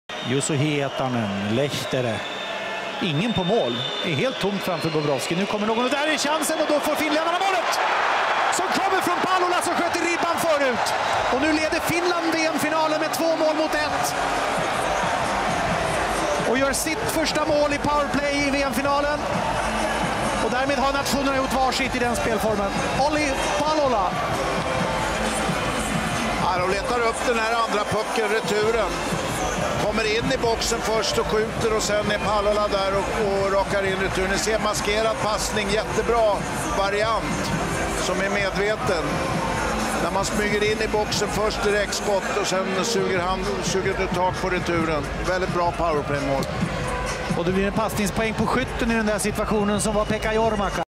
Russia vs Finland 2014-05-25 WC 2014 1-2 Goal
Source:TV4play
2014 IIHF World Championship in Minsk
there you can see all highlights in Russian in good quality.
!!!PLEASE DON*T MIX SPORTS WITH POLITICS OR VICE VERSA THO I GUESS SOMETIMES IT'S IMPOSSIBLE!!!